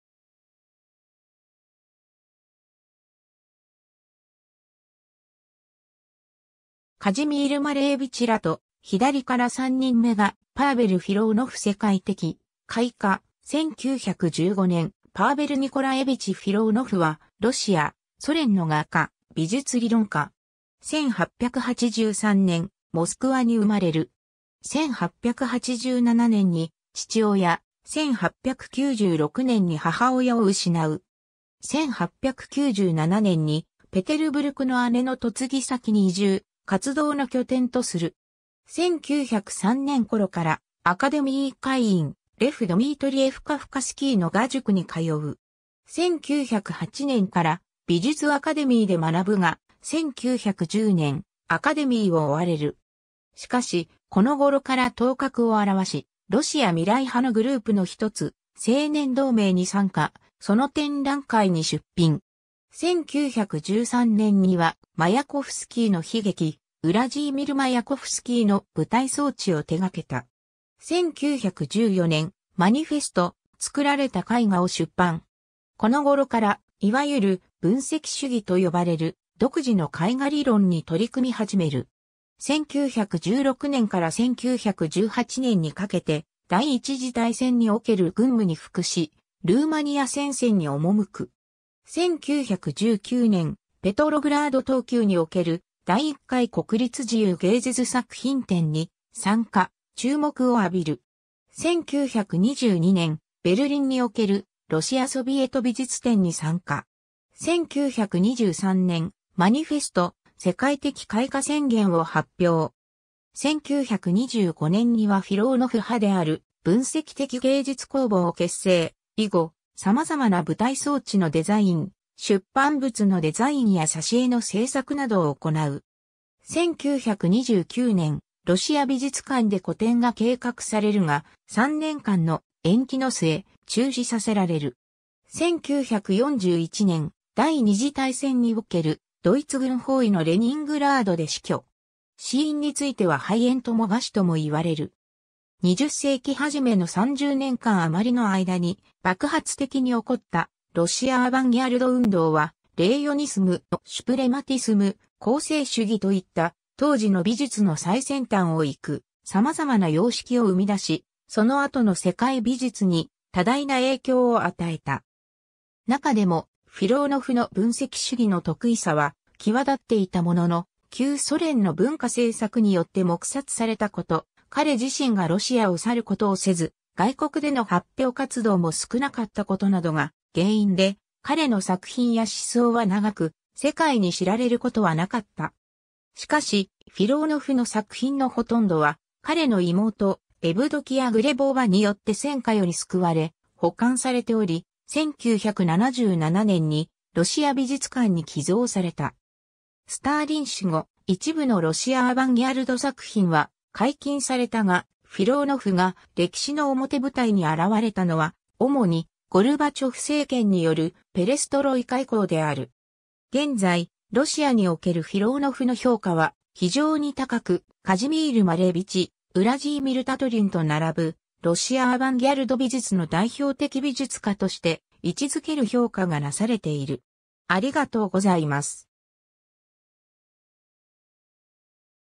パーヴェル・フィローノフ
ロシア・アヴァンギャルドの人物
カジミール・マレーヴィチらと 左から3人目がパーヴェル・フィローノフ 世界的開花, 1915年 パーヴェル・ニコラエヴィチ・フィローノフ（Павел Николаевич Филонов, Pavel Nikolayevich Filonov、1883年1月8日 - 1941年12月3日）は、ロシア、ソ連の画家、美術理論家。
 1883年、モスクワに生まれる。
1887年に父親、1896年に母親を失う。
1897年にペテルブルクの姉の嫁ぎ先に移住、活動の拠点とする。
1903年頃からアカデミー会員、レフ・ドミートリエフ＝カフカスキーの画塾に通う。
1908年から美術アカデミーで学ぶが、1910年、アカデミーを追われる。
しかし、この頃から頭角をあらわし、ロシア未来派のグループのひとつ、「青年同盟」に参加、その展覧会に出品。
1913年にはマヤコフスキーの悲劇「ウラジーミル・マヤコフスキー」の舞台装置を手掛けた。
1914年、マニフェスト「つくられた絵画」を出版。
この頃から、いわゆる「分析主義」と呼ばれる、独自の絵画理論に取り組み始める。
1916年から1918年にかけて、第一次大戦における軍務に服し、ルーマニア戦線に赴く。
1919年、ペトログラード冬宮（現在のエルミタージュ美術館）における「第一回国立自由芸術作品展」に参加、注目を浴びる。
1922年、ベルリンにおけるロシア・ソビエト美術展に参加。
1923年、マニフェスト「世界的開花宣言」を発表。
1925年にはフィローノフ派である「分析的芸術工房（МАИ／マイー）」を結成、以後、様々な舞台装置のデザイン、出版物のデザインや挿絵の制作などを行う。
1929年、ロシア美術館で個展が計画されるが、三年間の延期の末、中止させられる。
1941年、第二次大戦におけるドイツ軍包囲のレニングラードで死去（58歳）。
死因については肺炎とも餓死とも言われる。
 20世紀初めの30年間余りの間に、爆発的に起こったロシア・アヴァンギャルド運動は、レイヨニスム、シュプレマティスム、構成主義といった、当時の美術の最先端を行く様々な様式を生み出し、その後の世界美術に多大な影響を与えた。
中でも、フィローノフの分析主義の特異さは際だっていたものの、旧ソ連の文化政策（社会主義リアリズム路線の採択など）によって黙殺されたこと、彼自身がロシアを去ることをせず、外国での発表活動も少なかったことなどが原因で、彼の作品や思想は長く世界に知られることはなかった。
しかし、フィローノフの作品のほとんどは、彼の妹、エヴドキヤ・グレボーヴァによって戦火より救われ、保管されており、1977年にロシア美術館に寄贈された。
スターリン死後、一部のロシア・アヴァンギャルド作品は解禁されたが、フィローノフが歴史の表舞台に現れたのは、主にゴルバチョフ政権によるペレストロイカ以降である。
現在、ロシアにおけるフィローノフの評価は非常に高く、カジミール・マレーヴィチ、ウラジーミル・タトリンと並ぶ、ロシア・アヴァンギャルド美術の代表的美術家として位置付ける評価がなされている。